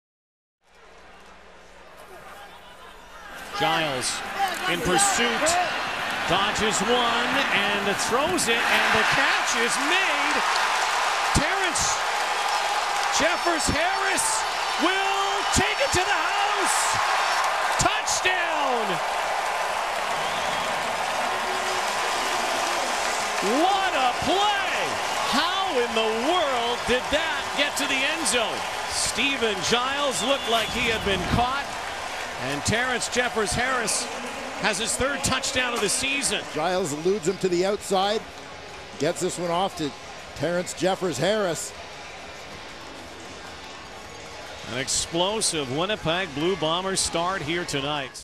CFL T.J. Harris Catch & Run TD
Steven Jyles sheds a tackle and gets the ball to Terence Jeffers-Harris who somehow stays on his feet and runs the ball in for a touchdown.